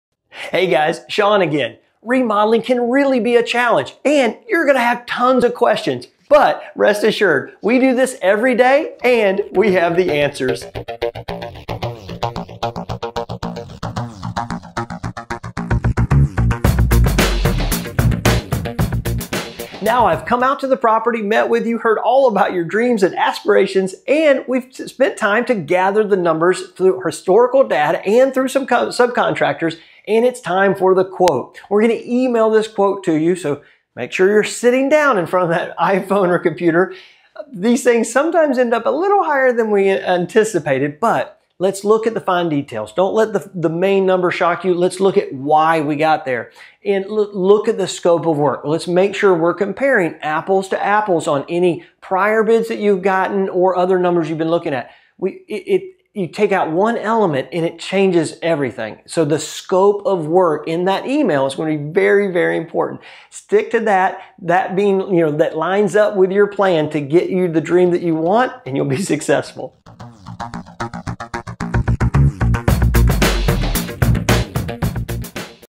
Home Remodel Timeline: Ep. 3
Follow along with us as we go step by step through the home remodel process! Step 3: Receive your quote!